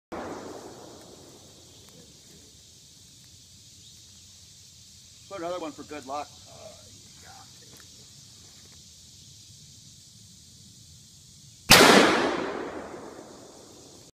Gun Rights Redneck Veteran with Smith & Wesson 500 Magnum in junk yard
I created this video for pure entertainment.